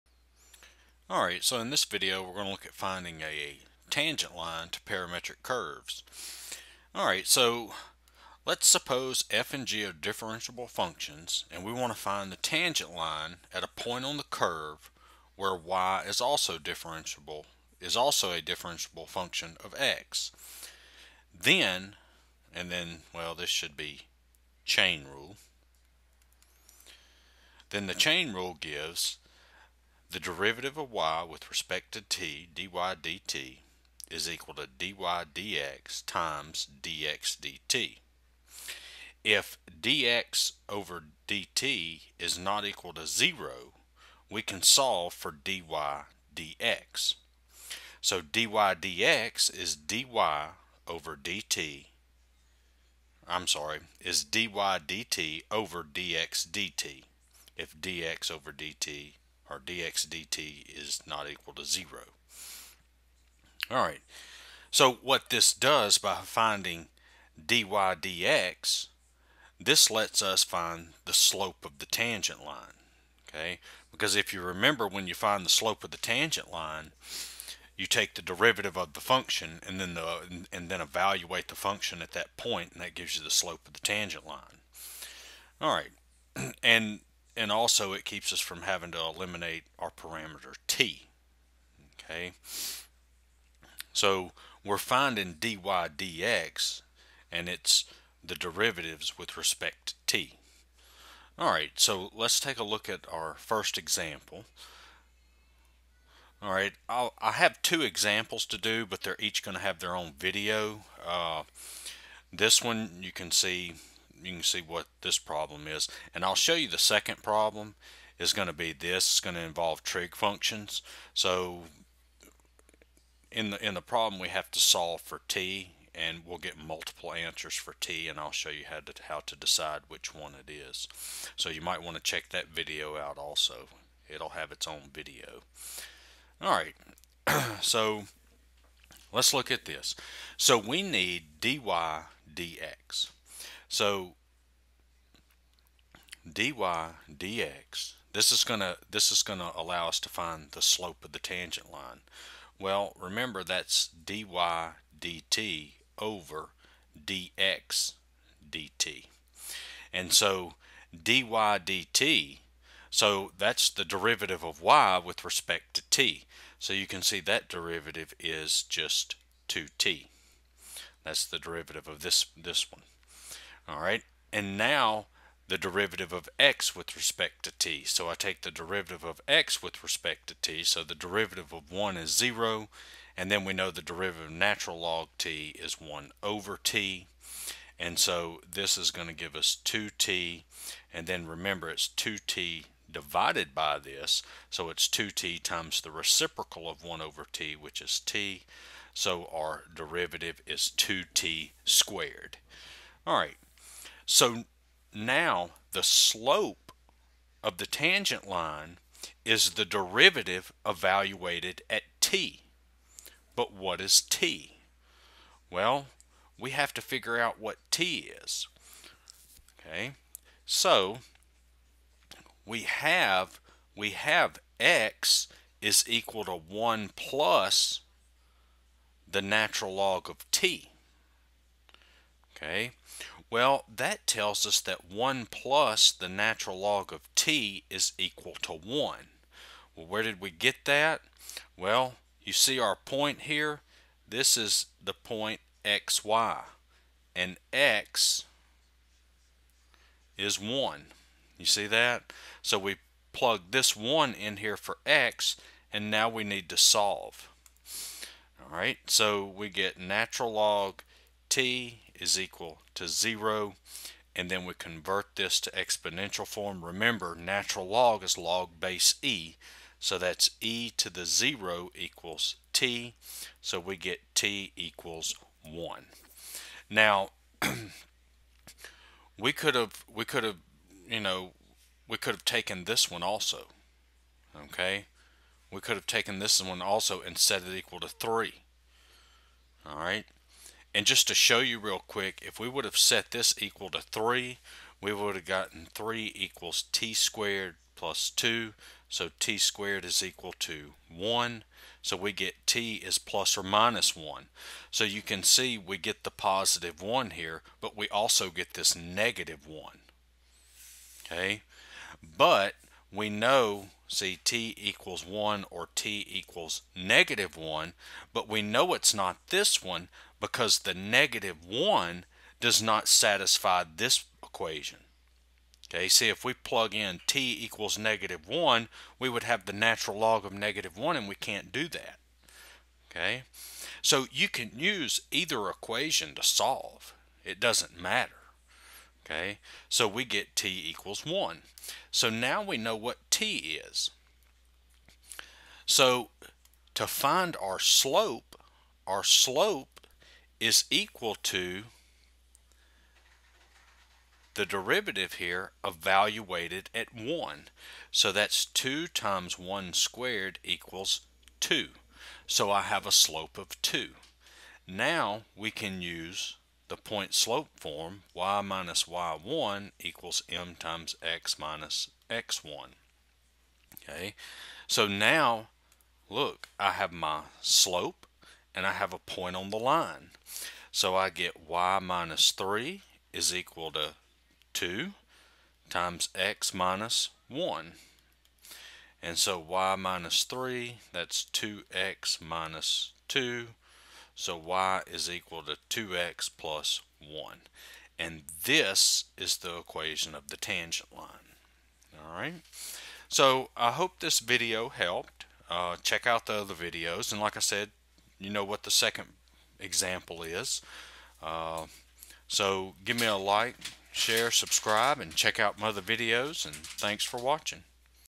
Find Equation of The Tangent Line To The Parametric Curve Example 1
This video shows how to find the equation of the tangent line given parametric equations.